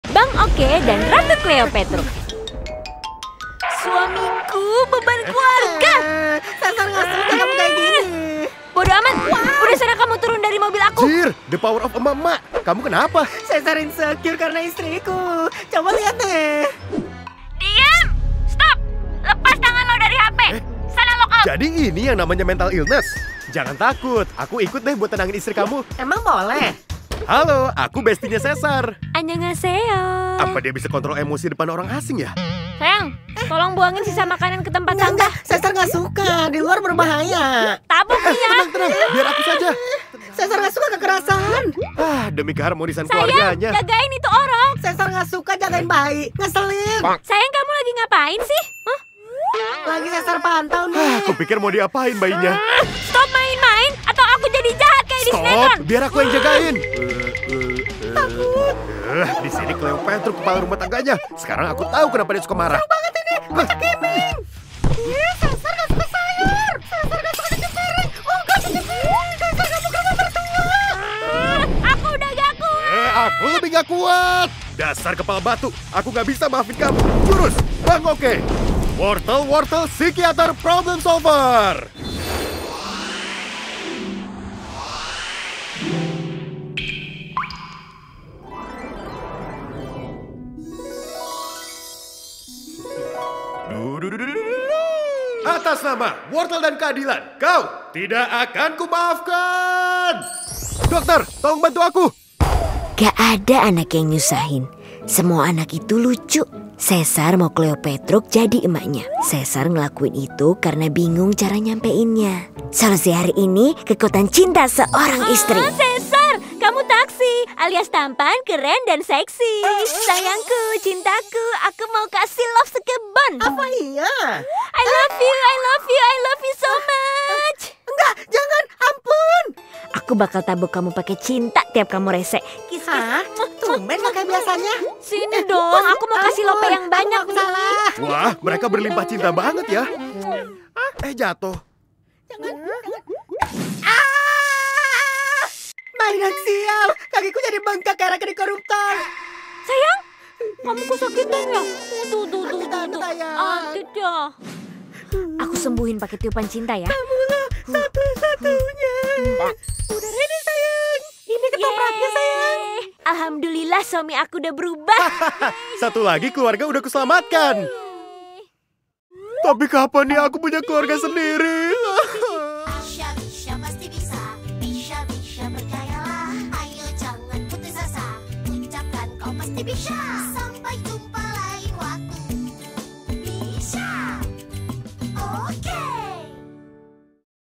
Cleopetruk Lebih Sayang Caesar Setelah Dibantuin BangOK
Caesar akhirnya dibantuin BangOK supaya gak kena mental gara-gara dimarahin terus sama Cleopetruk 😢 Tonton langsung videonya biar gak penasaran yuk! 🤩

Ngomong-ngomong BangOK mau spill nih pengisi suara video Cleopetruk adalaaaah @tissabiani suaranya keren banget buat isi karakter Cleopetruk ya✨🤩

Seru banget BangOK punya kesempatan kerja sama bareng kak Tissa, kamsahamnida Kak!🫰

Btw, kalian ada yang mau titip salam ke kak Tissa gak nih? 😝